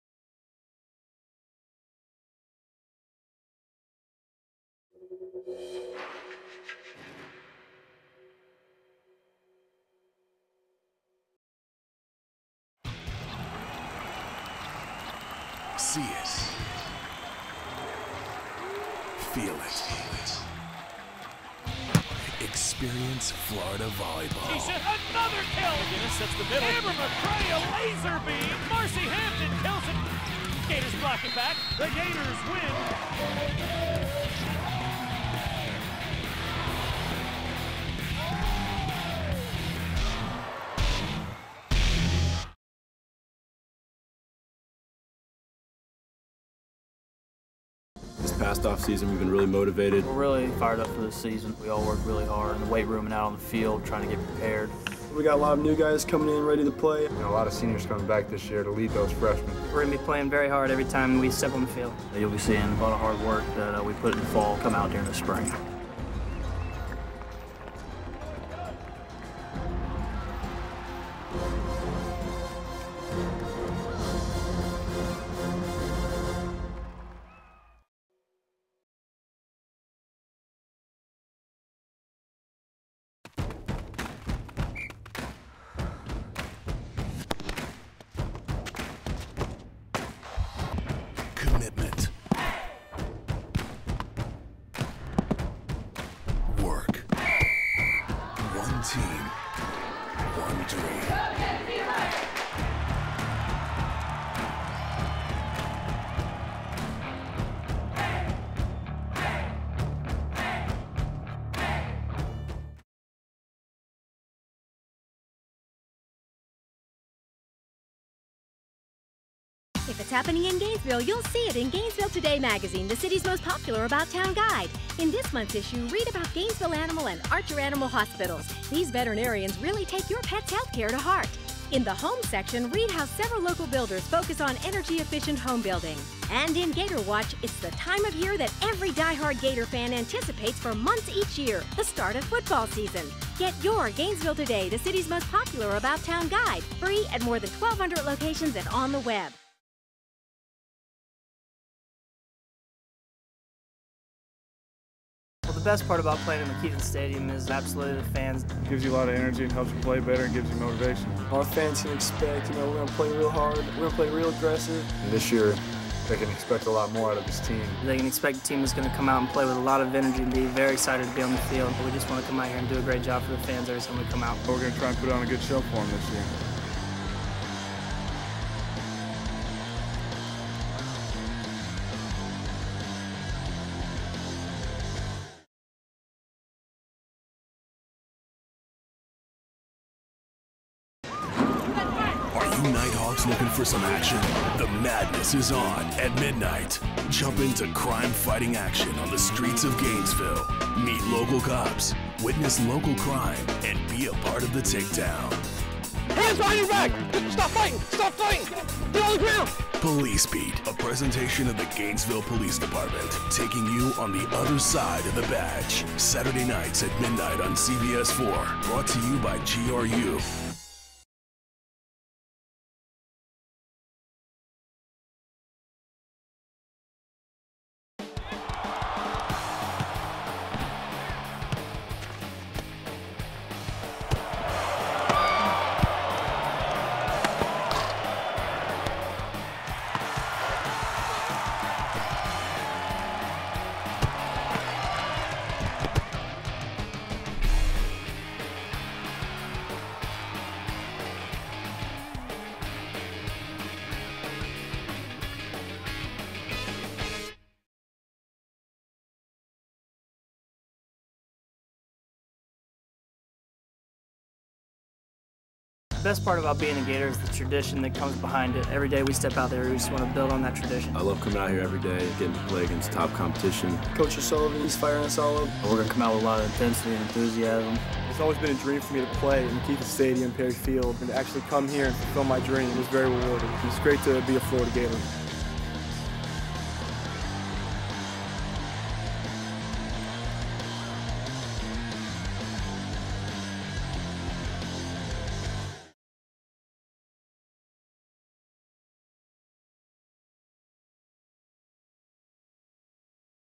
Commercial Production Video Editing Reel by Brenton Richardson
This video is meant to demonstrate the commercial production and video editing skills of Brenton Richardson.

These commercials were produced by Studio 601 Inc. between 2007 and 2009 in Gainesville, Florida.